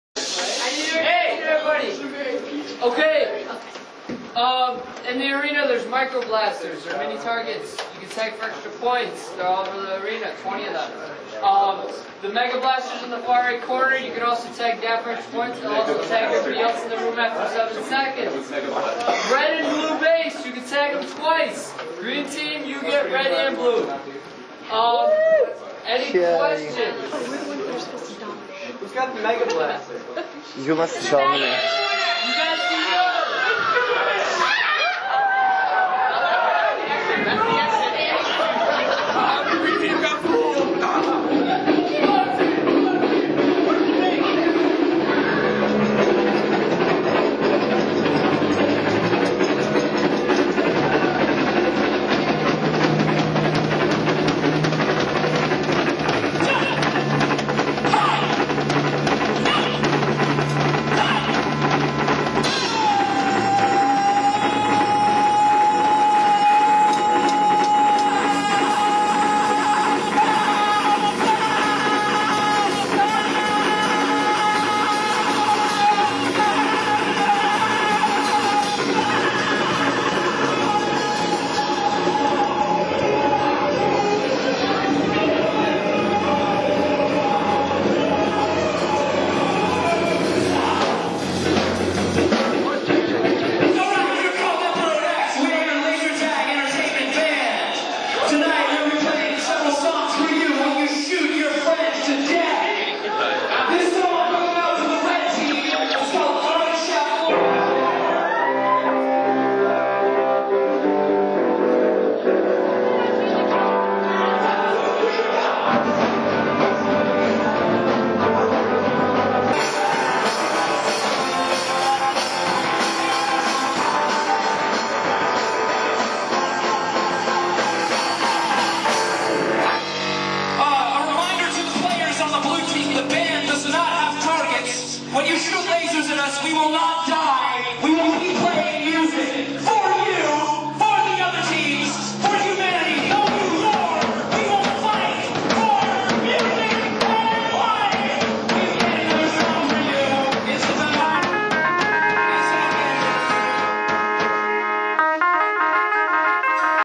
Emperor X -- Live in a Connecticut Laser Tag Arena
Videographer/booking: Lee Pender